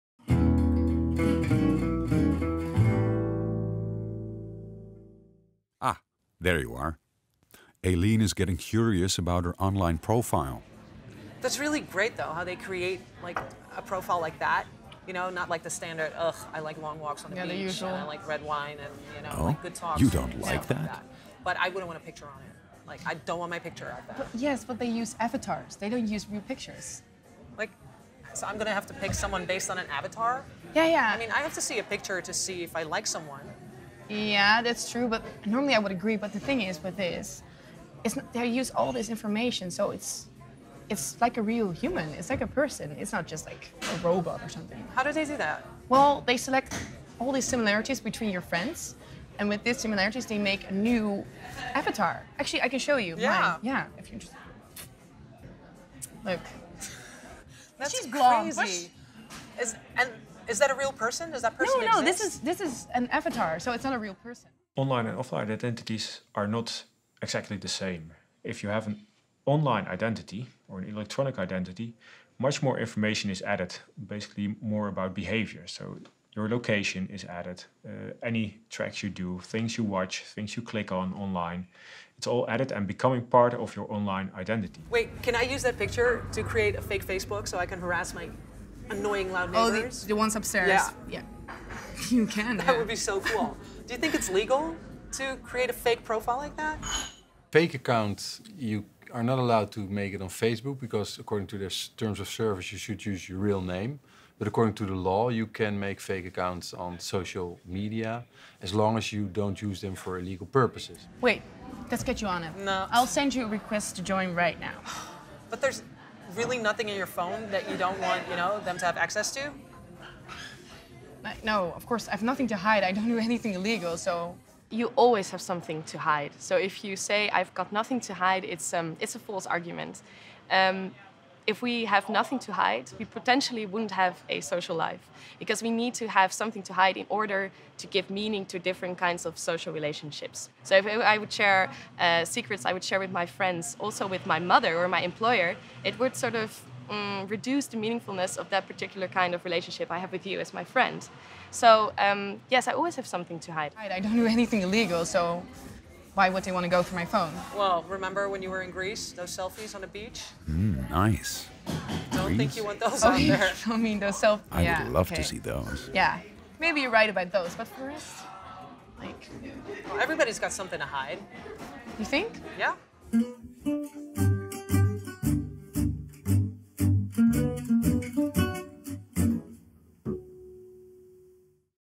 The things we do for love (eps. 2 - 5)
WHO ARE YOU TODAY?
It is in our nature to constantly ask ourselves 'Who am I?'. We present ourselves with friends, colleagues, family, to get feedback on this presented version of ourselves. With this feedback we create our new self, our new identity and say to the world 'This is who I am' and get feedback again. We go through this loop continuously *.
Depending on the feedback we got, we present ourselfves differently to different people in different environments. You are probably a different person with your mother in law then with your own mother. You might be know for your enthousiasm with your friends but for your professionalism at work.

If we only had one presentation of ourselves, one identity, we wouldn't be able to maintain all these different people and different environments as we did before.  Imagine how boring our lives would be. This is what would happen if we give away all of our personal data.

created by Panos Kostouros & Ilja van den Berg

A MINI-SERIES ON CYBER SECURITY
According to criminologist Marleen Weulen Kranenbarg Online criminals are much more intelligent than the traditional ones.  It is not enough to protect ourselves with firewalls and antivirus software. We need to be much more aware of our online behavior. Therefore we created this online mini-series,